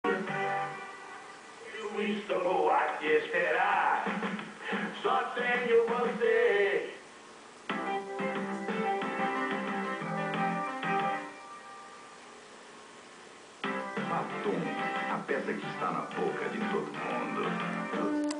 Ana Paula Arósio - Peça Batom - 1995
Comercial de divulgação da peça Batom, sendo a estréia da Ana Paula nos palcos